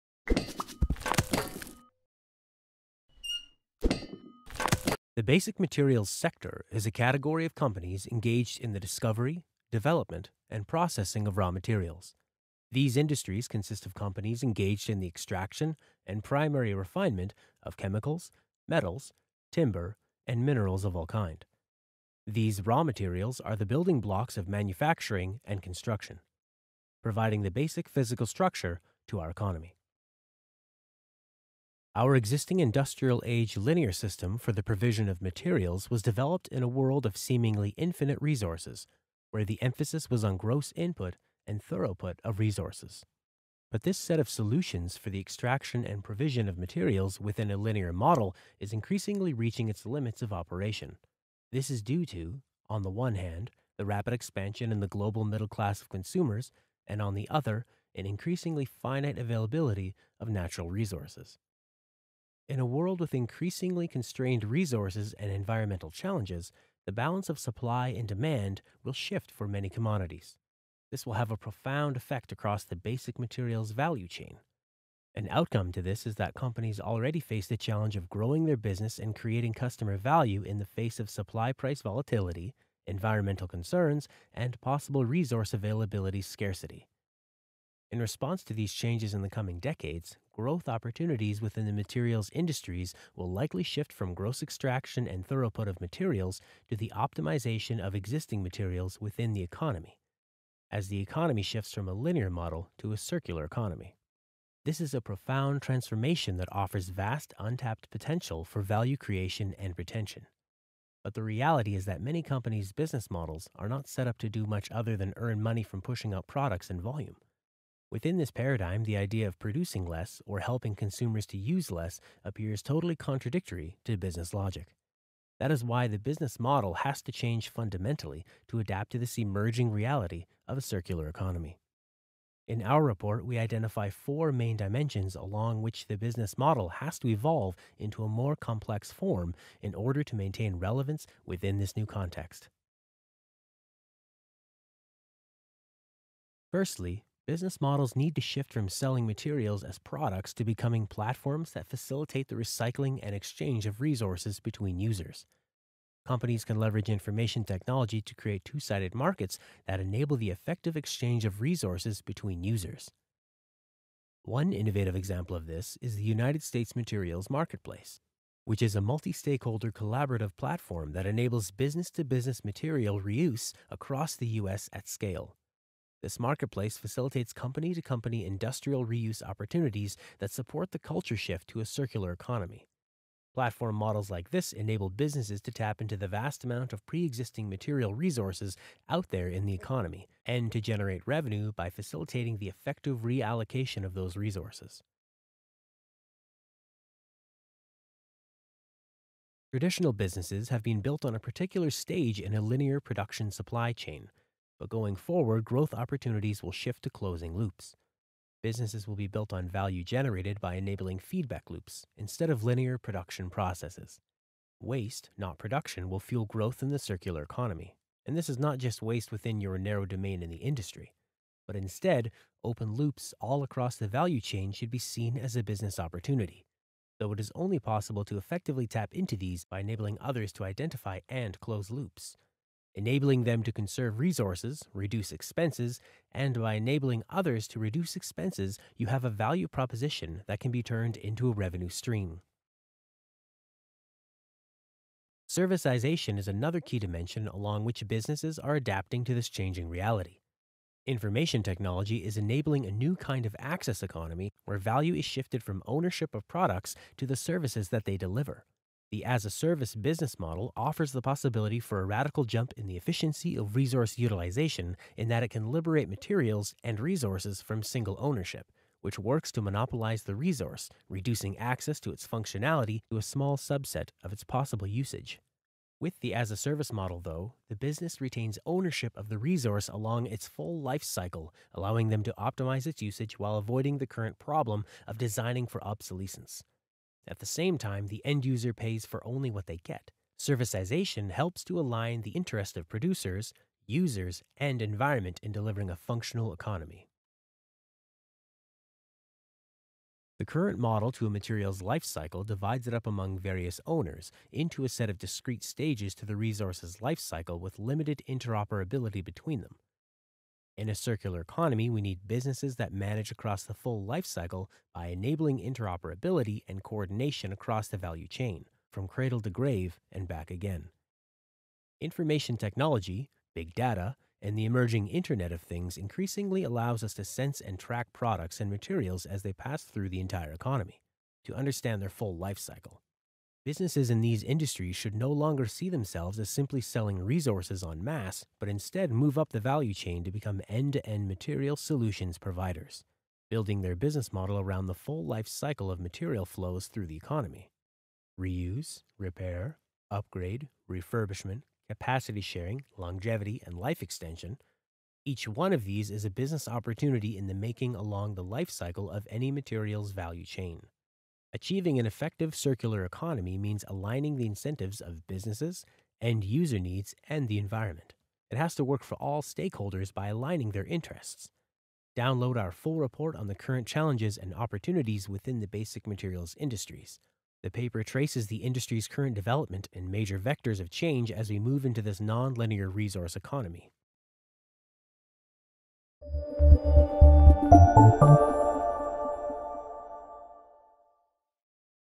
Basic Materials Research
A short video giving an overview of our circular economy solutions for businesses in the basic materials sector including mining, forestry, chemicals, and commodities.